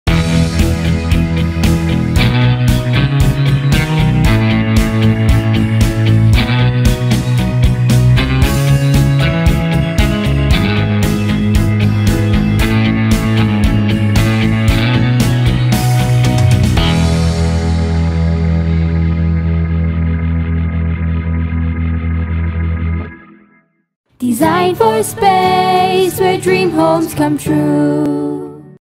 [Singapore Interior Design] A Full Makeover to 20 Years Old Resale Flat
Take a look at what we’re doing to makeover a 20 Years Old home.

Designed by Shermin Lee from Design 4 Space

Project Details:

Interior Theme:
Modern Luxury

Location:
447 Choa Chu Kang
Choa Chu Kang, West region in Singapore.

Homeowner: Family of 4
Project Condition: Resale Flat
Project Type: HDB 4 Room
Estimate Renovation period: 12 weeks

Essential Client’s Requirements:
Color Scheme

Works:
Hacking
Masonry
Vinyl flooring
Painting
Plumbing
Door Works
Glassworks
Carpentry
Table Top
Electrical
Fluted Panel.